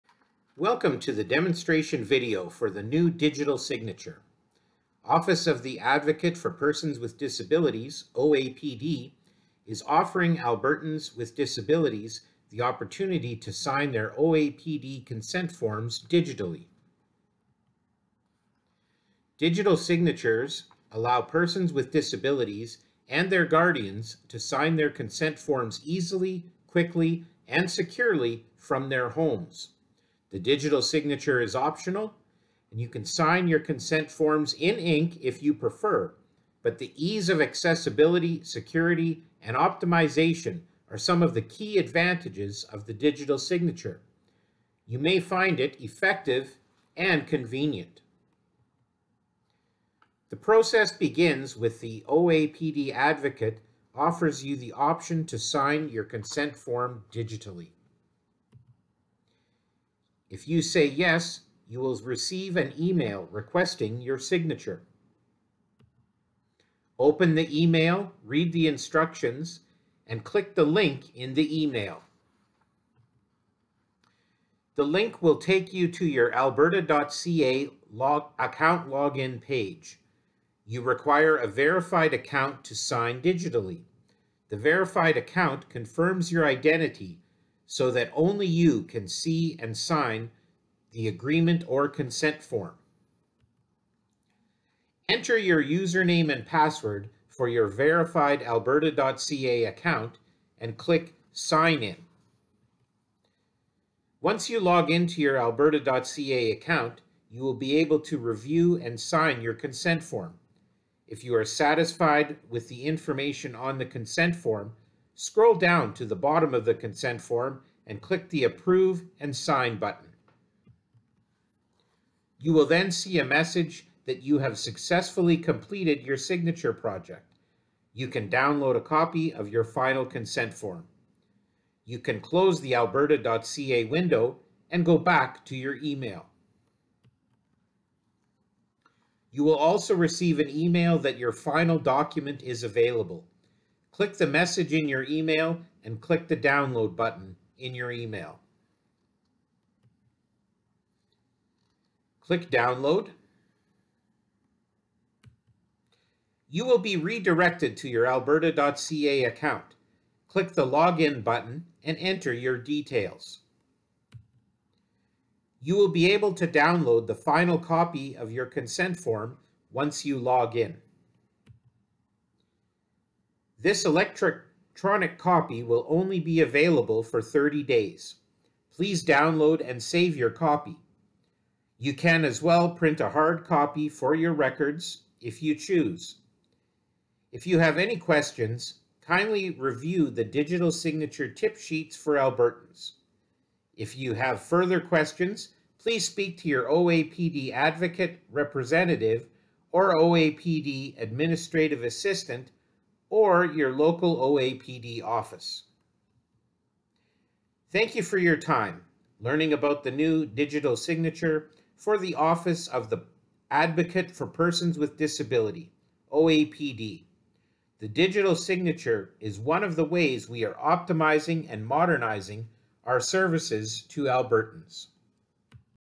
OAPD Digital Signature Registration Demonstration
A demonstration by the Office of the Advocate for Persons with Disabilities (OAPD) on how Albertans will be able to use a digital signature and sign OAPD consent forms digitally.